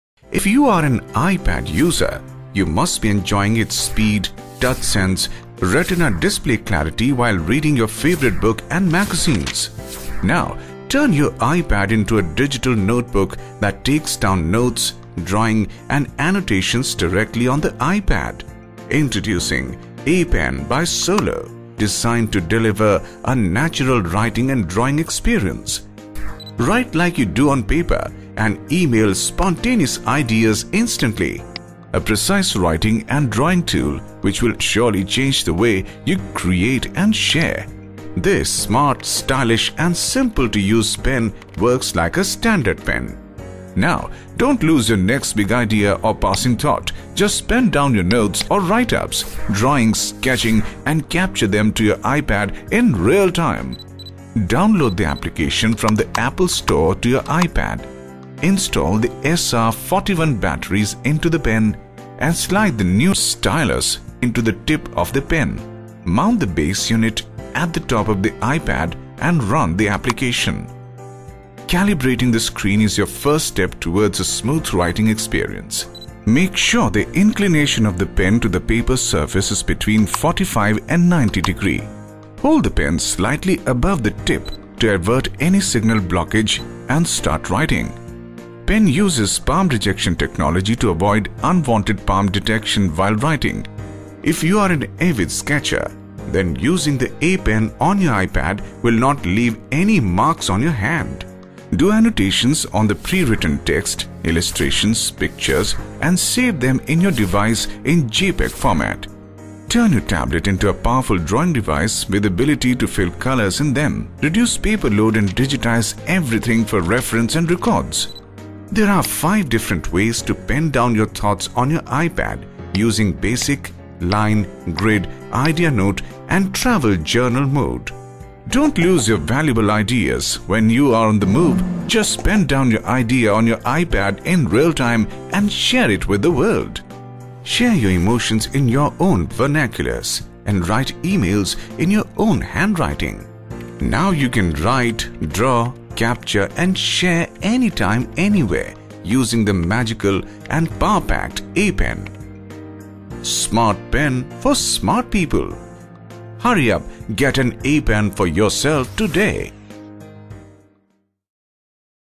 Stationery Products - Onscreen Digital Pen For iPad
(a) Enhance your ipad experience . Now make it your notebook to write emails or     handwritten letters.
(b) Dongle type receiver, plug in and write freely on ipad screen .
(c) You can lean your palm on capacitive screen during writing, very user-friendly .
(d) Input very accurate and fine drawing and writing without changing your writing e-mails, Math, Physics, Chemistry formulas, graphic, diagrams can be input on ipad screen freely.
(e) Do photo sketcher on JPG pictures on ipad screen.
(f)  Do annotation or e-signature on screen shot.
(g) Turn your ipad into a powerful drawing tablet.
(h) Paperless solution, green products.
(i) Free application is available on Apple's APP Store.
(j) Unique palm rejection technology that enables only input                                          (s) from 'apen' as soon it's tip is pressed on ipad screen.